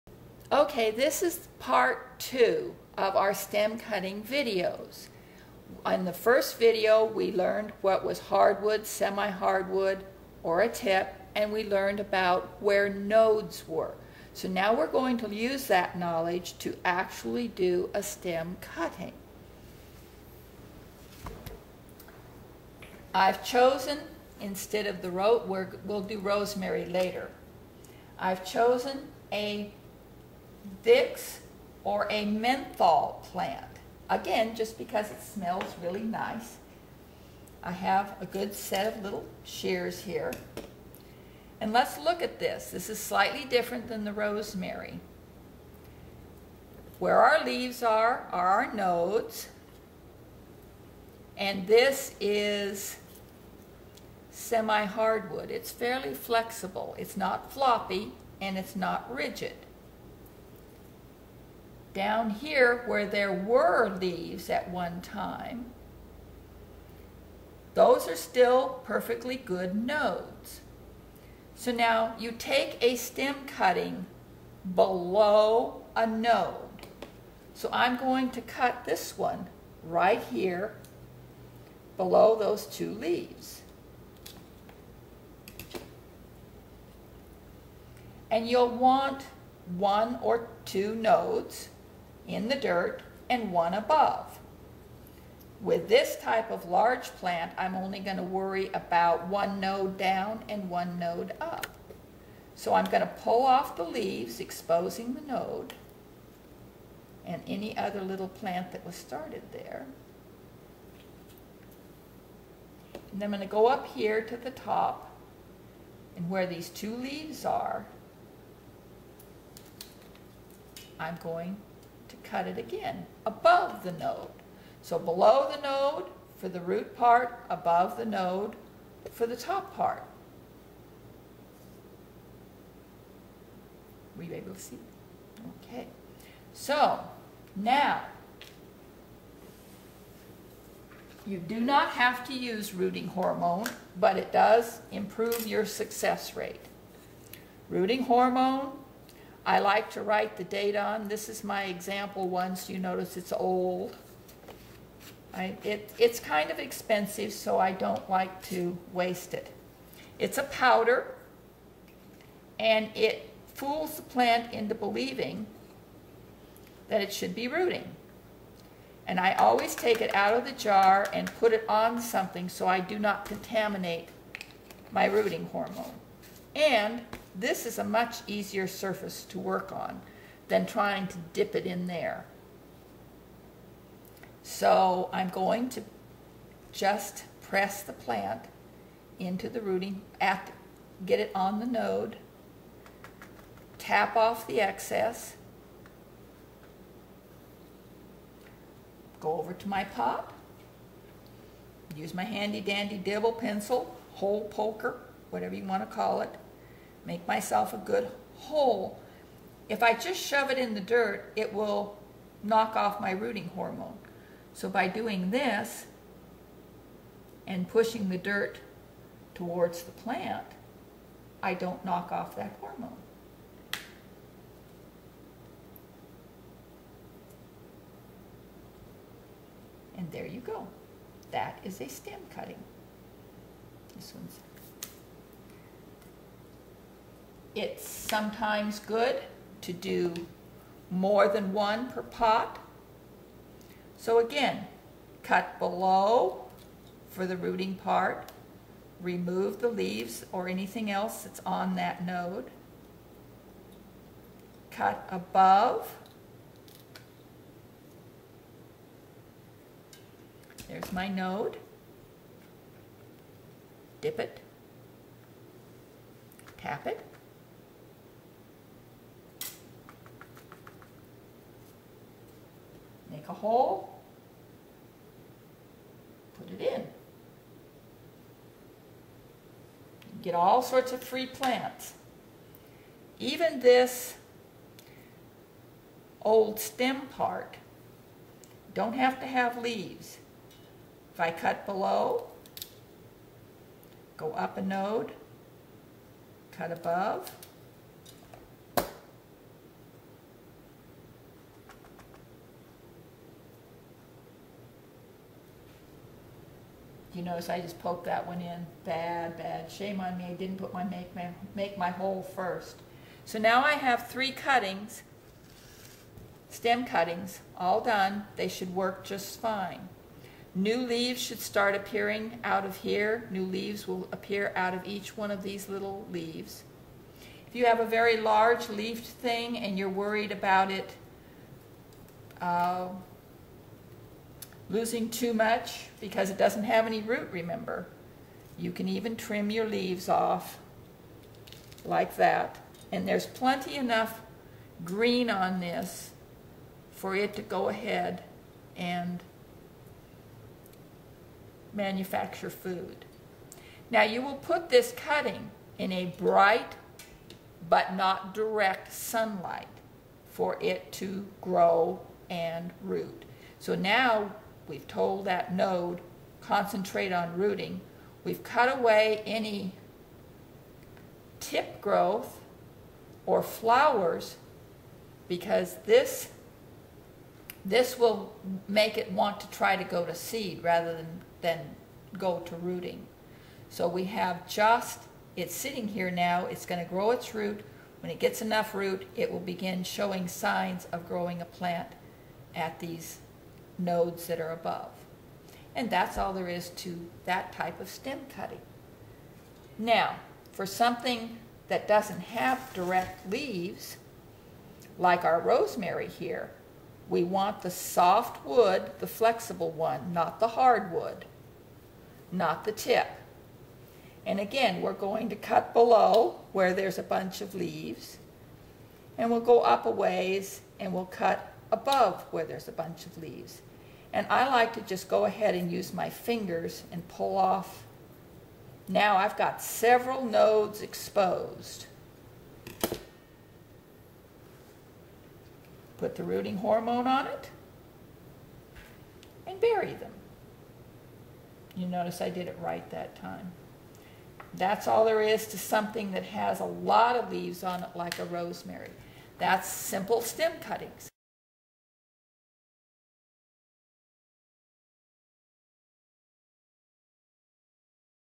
Stem cuttings, Part 2
Plant propagation through stem cuttings, part 2: Preparing and planting the cutting.